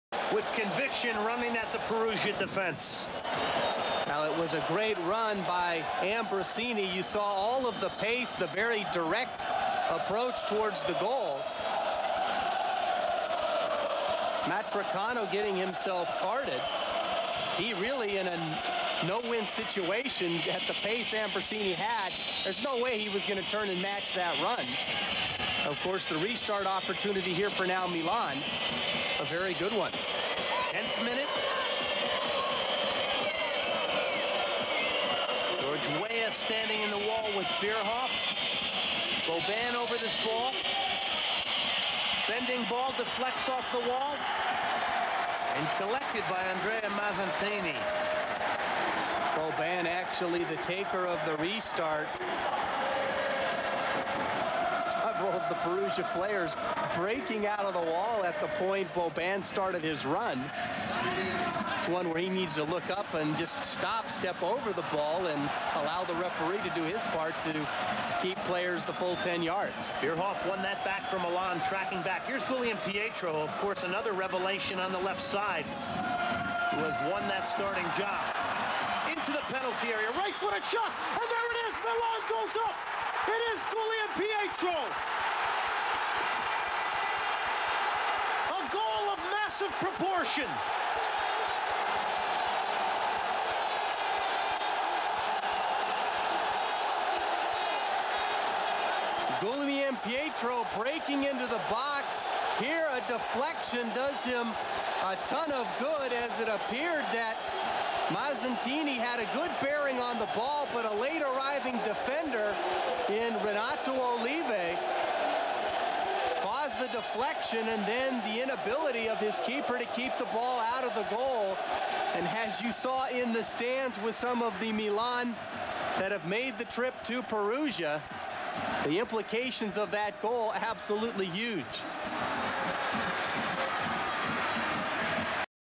Guglielminpietro goal over Perugia 1999
It feels like it was just last week that this all happened (: I do remember vividly how I celebrated this win with my then girlfriend.  I'm upset I couldn't find clips from our last Scudetto win back in 2004 but I figured this would still be a treat.  Andres Guglielminpietro goal on the 11th minute of the first half

AC Milan:
                       Abbiati
Helveg  Sala  Costacurta  Maldini
Albertini  Boban*  Ambrosini  Guglielminpietro
                 Weah  Bierhoff

*((Leonardo subs in for Boban in the 79th min))

AnGe|O

PS: please excuse the video quality, it originated from a VHS